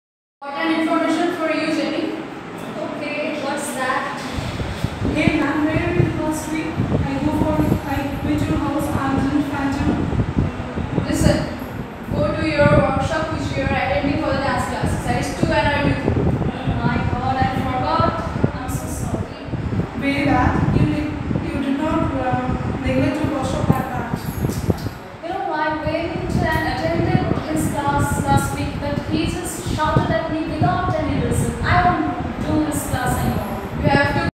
Spoken English Training
Spoken English training with Audio Visual Aids in my institute